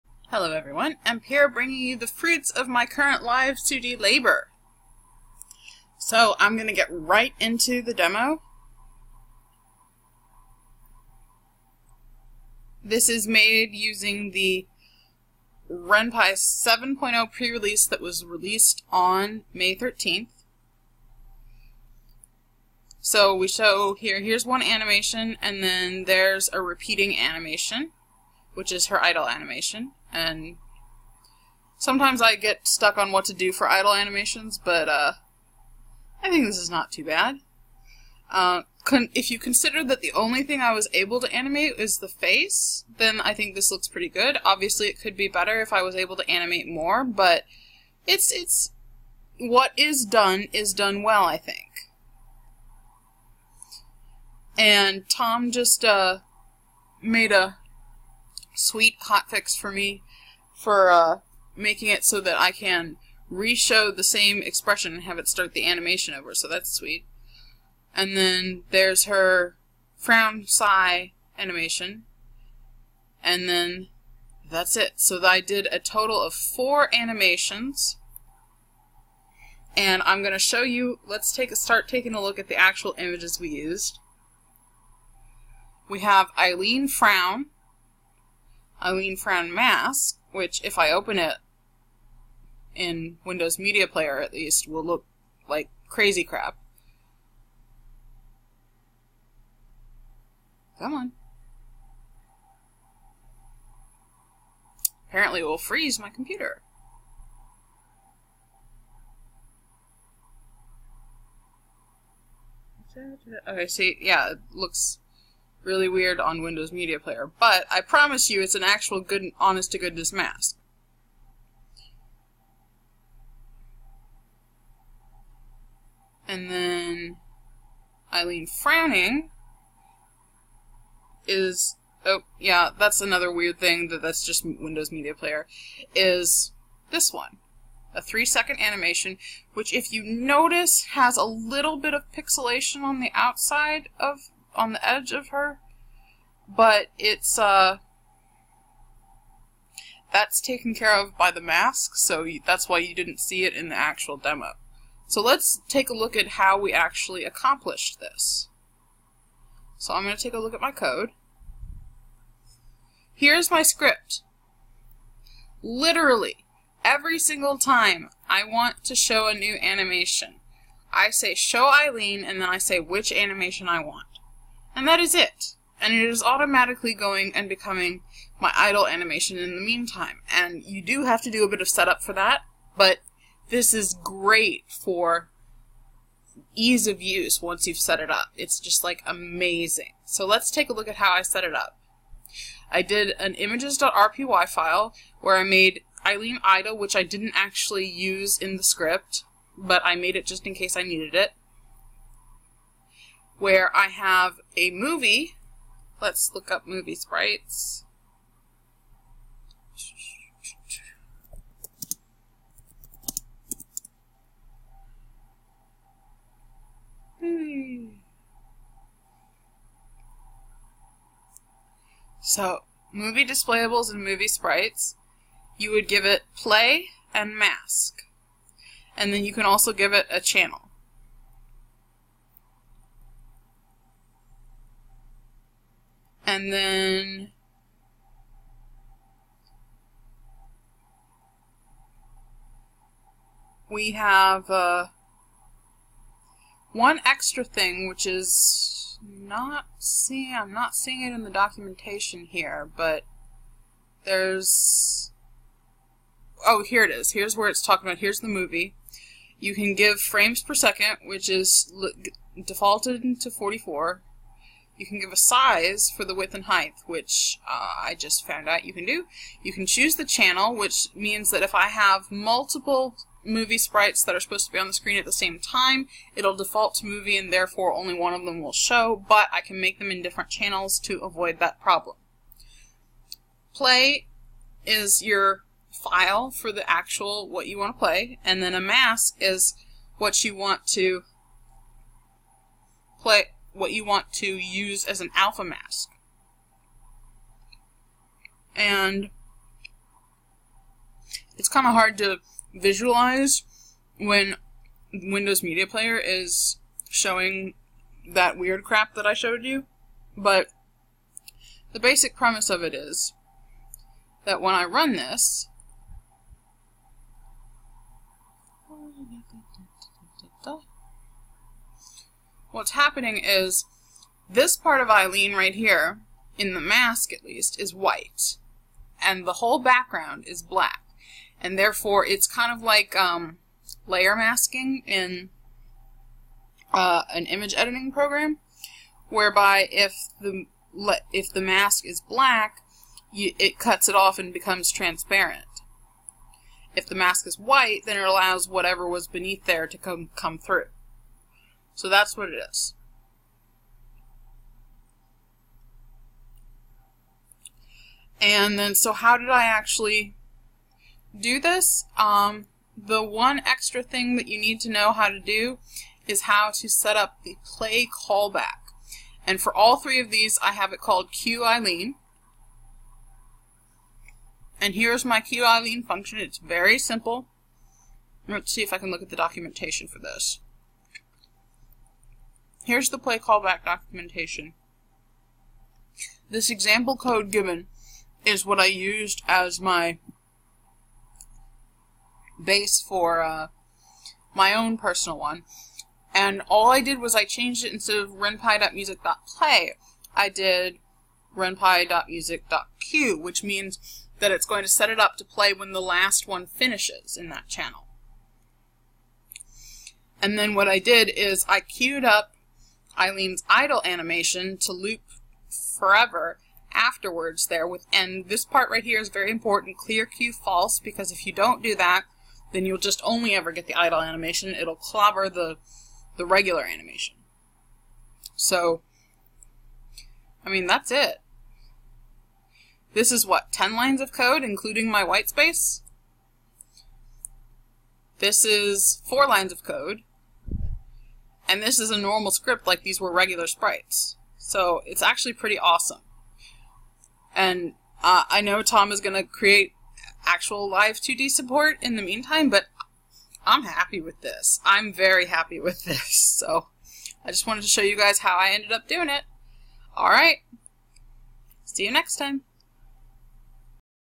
Eileen's Live2D Adventure Continues!
In which I show you just how EASY it is to implement your movie sprites in Ren'Py.